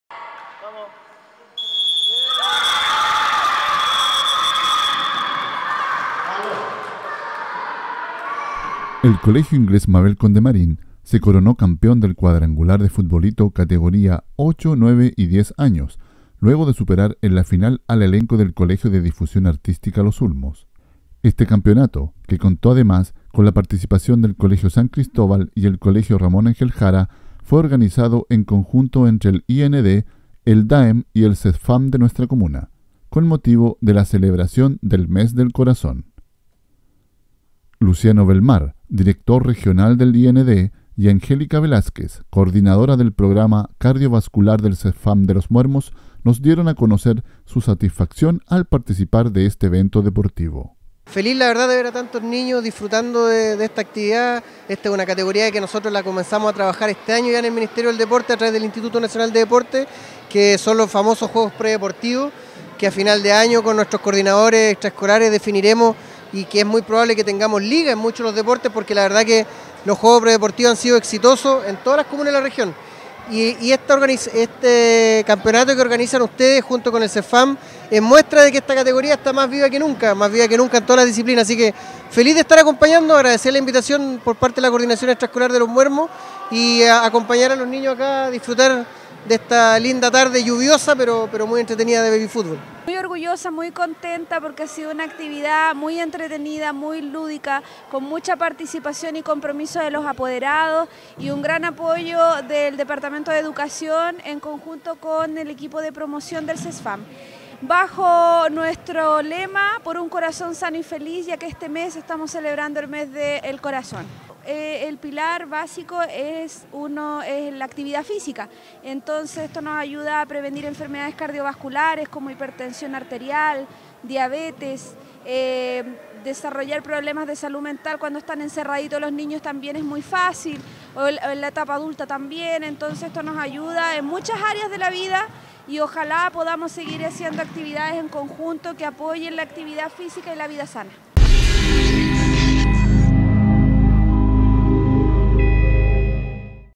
Final Futbolito Escolar 2015 - Mes del corazón
El Colegio Inglés Mabel Condemarín, se coronó campeón del cuadrangular de futbolito categoría 8-9-10 años.
Este Campeonato fue organizado en conjunto entre el IND, el DAEM y el CESFAM de nuestra Comuna, con motivo de la celebración del "Mes del Corazón" y que además contó con la presencia del Director Regional del IND don Luciano Belmar.
El cuarto puesto fue para el elenco del Colegio San Cristobal, el tercer puesto para el Colegio Ramón Angel Jara y el segundo lugar para el C.D.A Los Ulmos.